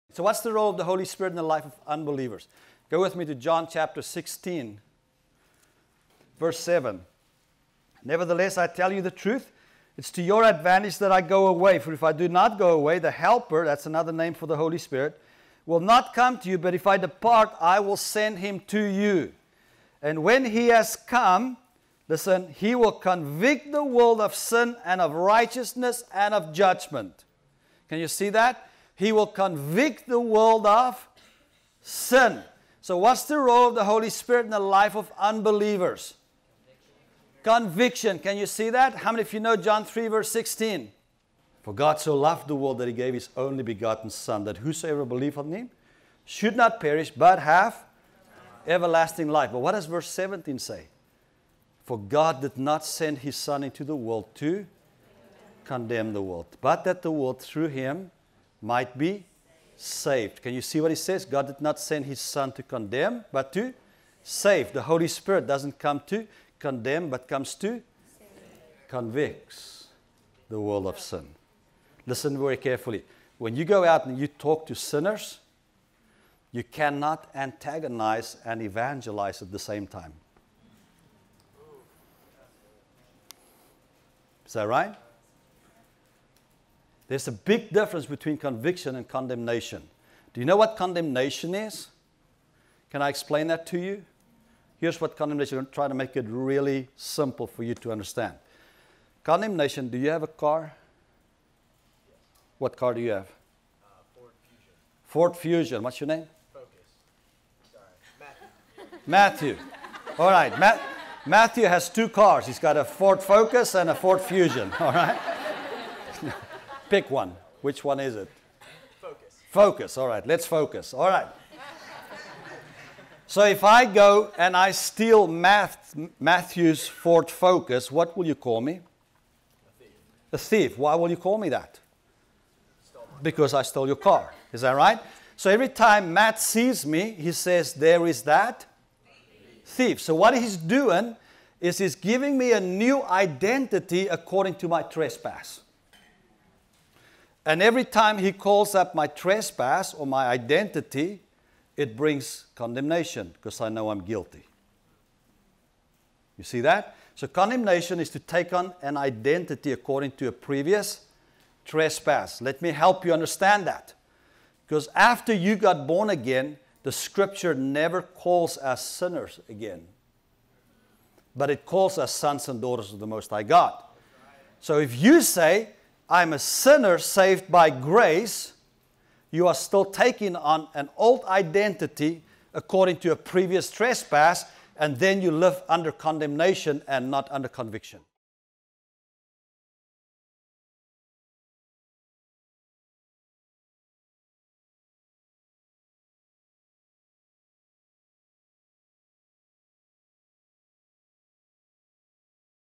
What is the role of the HOLY SPIRIT? Part Three - Deon Vanstaden - World Race Training Camp
Deon Vanstaden talking about the role of the Holy Spirit at World Race Training Camp.